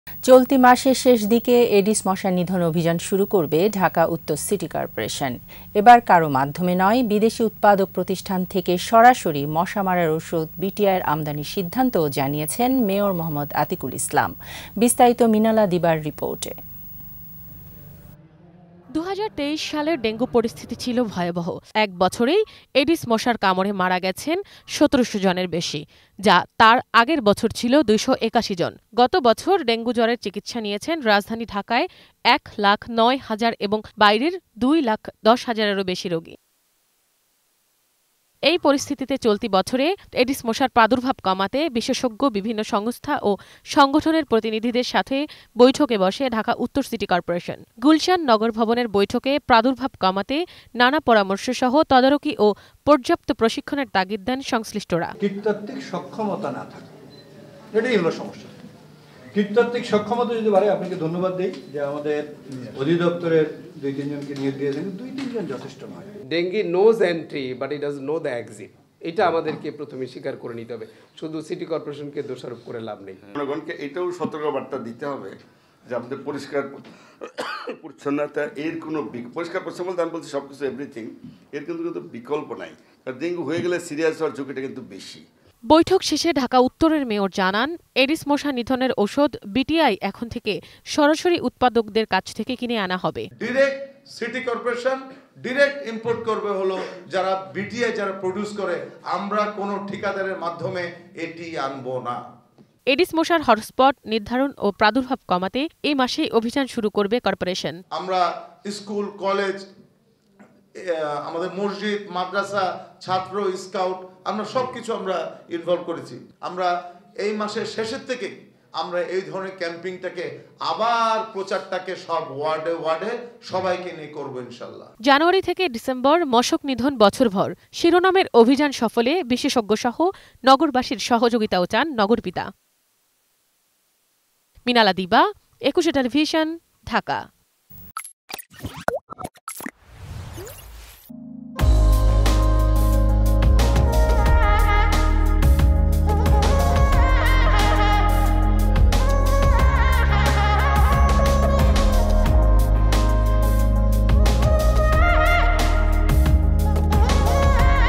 এডিস মশা নিধন অভিযান শুরু করবে ঢাকা উত্তর সিটি কর্পোরেশন || ETV News
চলতি মাসের শেষদিকে এডিস মশা নিধন অভিযান শুরু করবে ঢাকা উত্তর সিটি কর্পোরেশন। এবার কারো মাধ্যমে নয়, বিদেশি উৎপাদক প্রতিষ্ঠান থেকে সরাসরি মশা মারার ওষুধ বিটিআই আমদানির সিদ্ধান্তও জানিয়েছেন মেয়র মো. আতিকুল ইসলাম। বিস্তারিত মিনালা দিবার রির্পোটে।